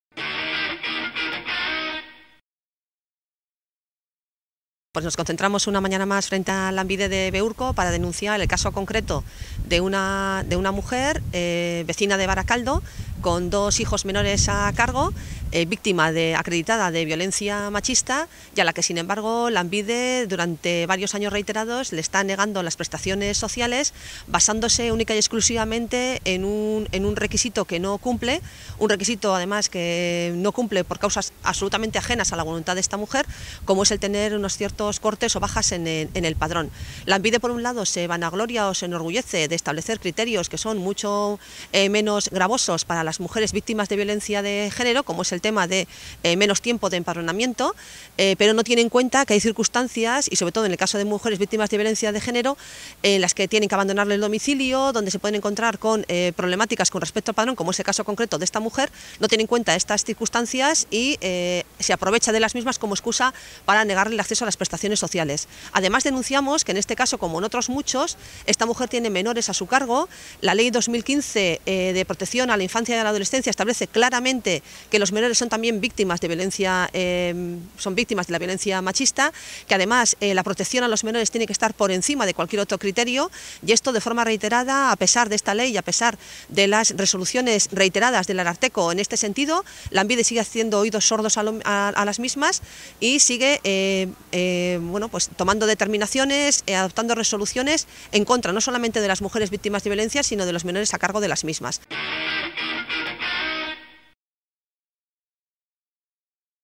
Denuncian las trabas de Lanbide a las mujeres que sufren violencia machista
Superheroina han denunciado la carrera de obstaculos que supone para las mujeres victimas de violencia machista el acceso a las prestaciones sociales en Lanbide.